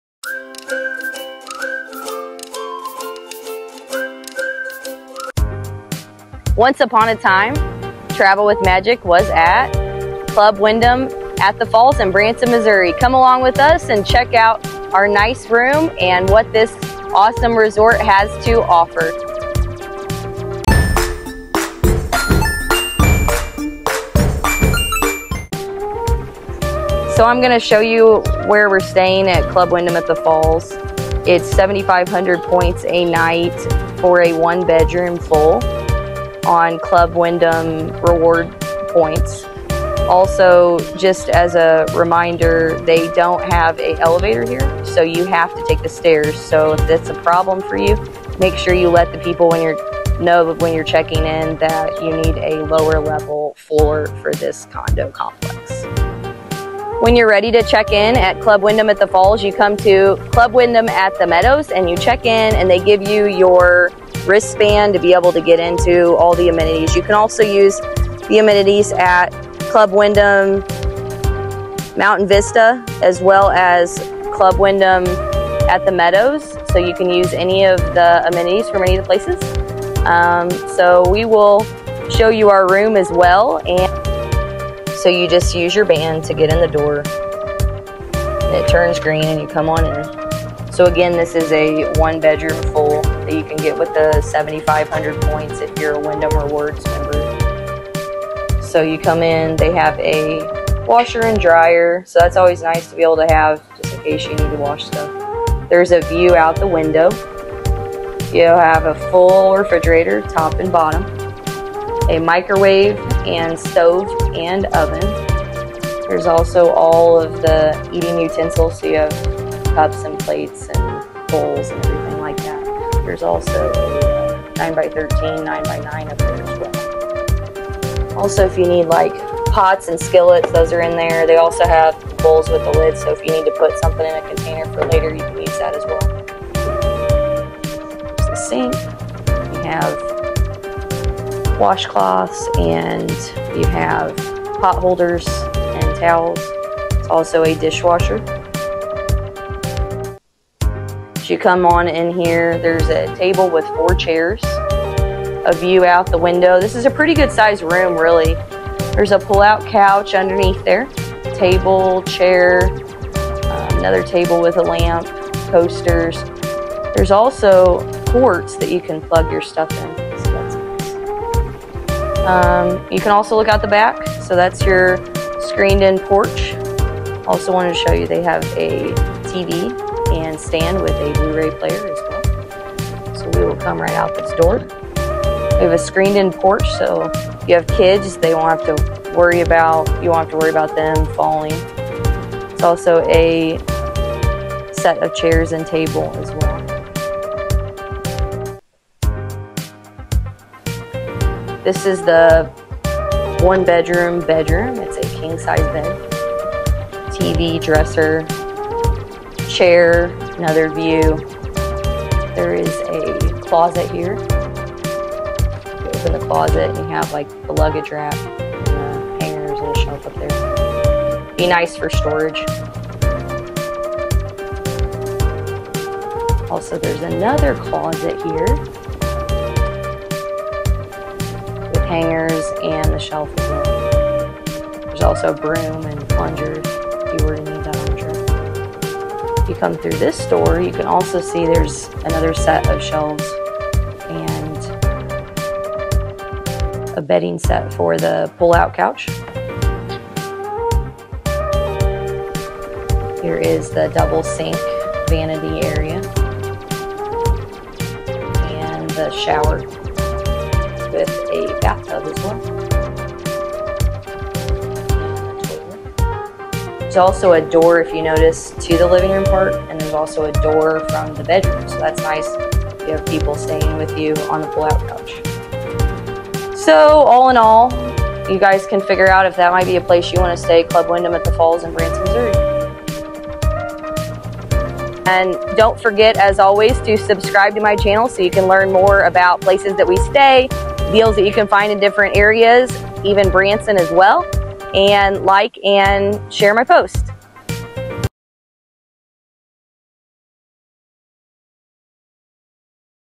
Branson Club Wyndham at the Falls | Travel With Magic
Are you interested in staying at a condo on your next Branson trip? Club Wyndham at the Falls could be the perfect place! Come check it out with us!